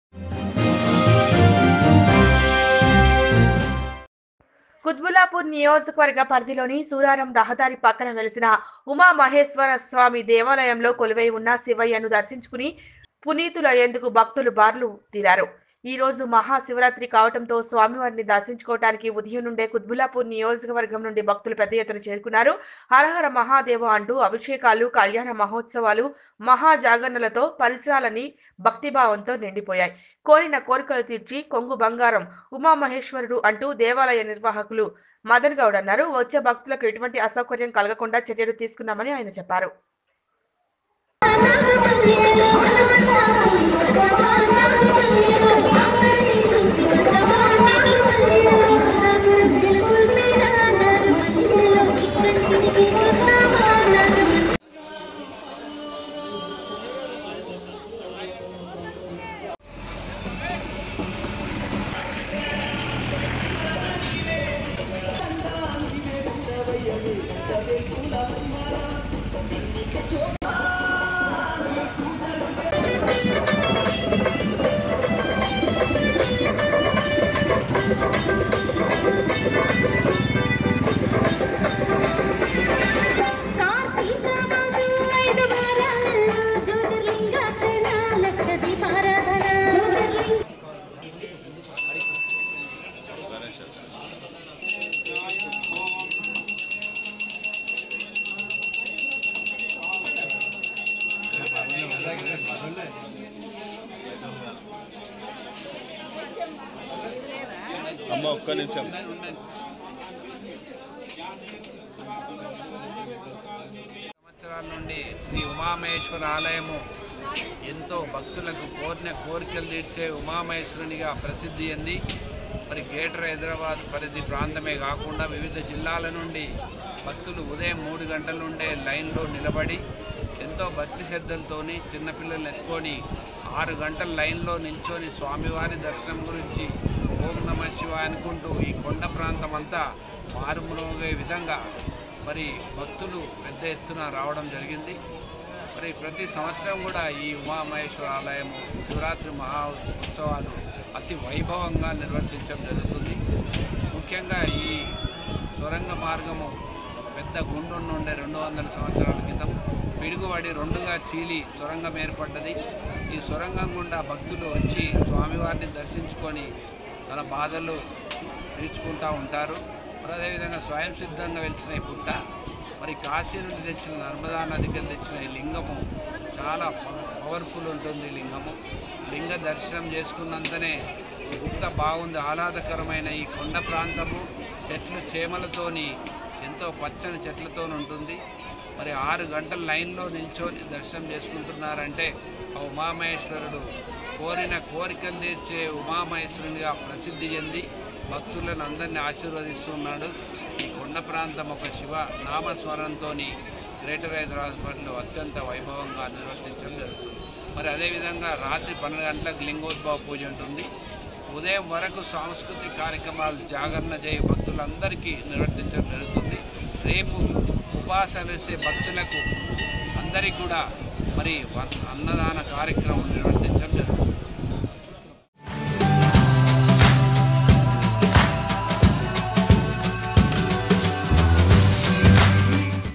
కుత్బుల్లాపూర్ నియోజకవర్గ పరిధిలోని సూరారంలో ఘనంగా మహా శివరాత్రి వేడుకలు |Mahasivratri | Nethra News
| Nethra News | Mahasivratri || Suraram |